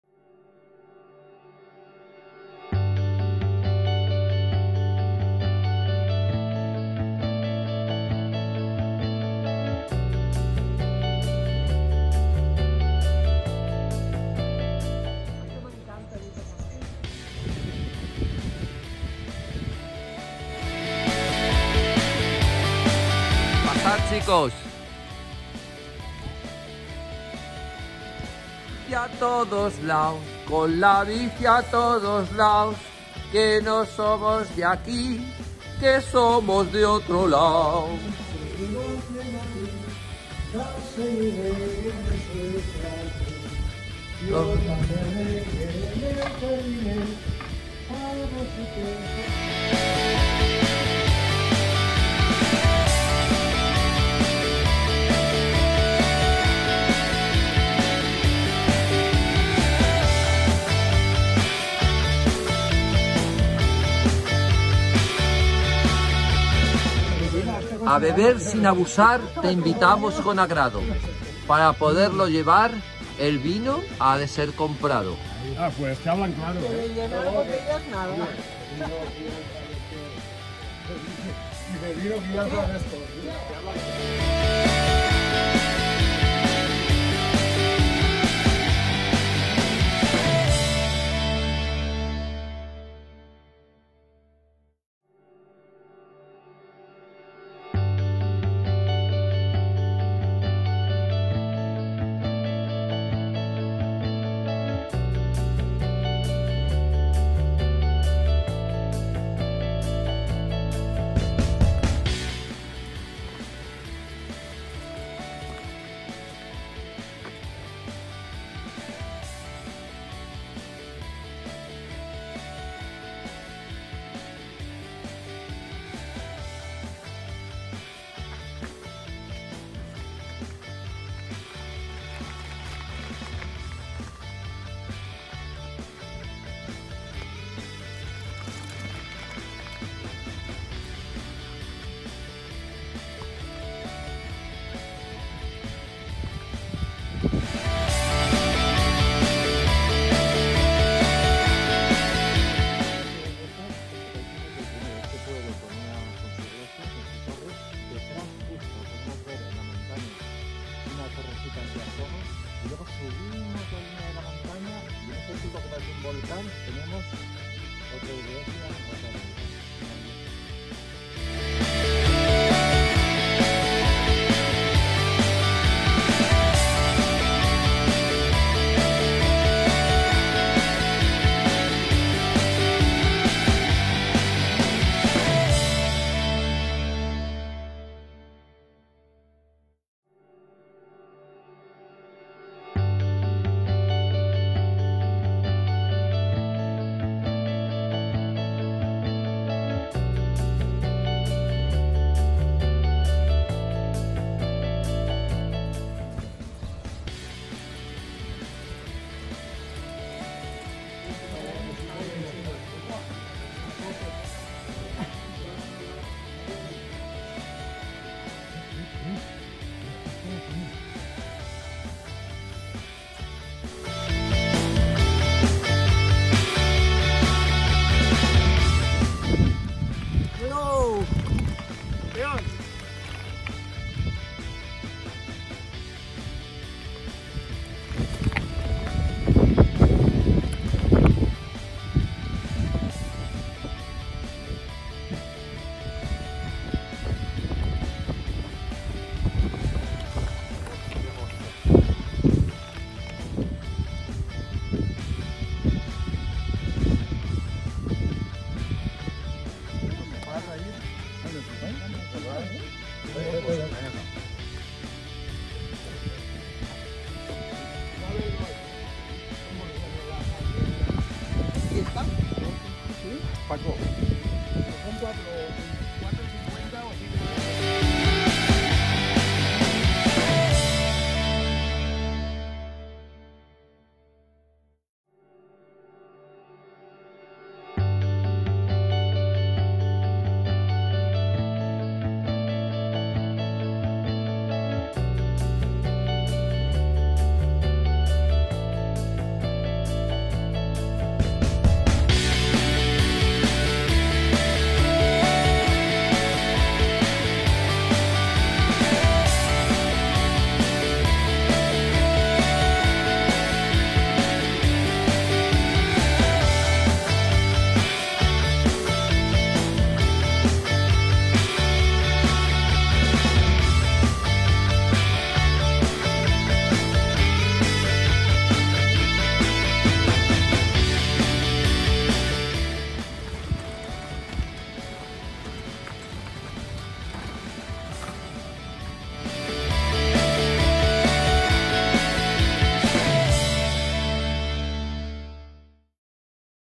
4 Etapa Estella- Logroño Camino De Santiago Frances en bici
Cicloturismo en Bicicleta  sosegado, poético y sensual. con Paco Tortosa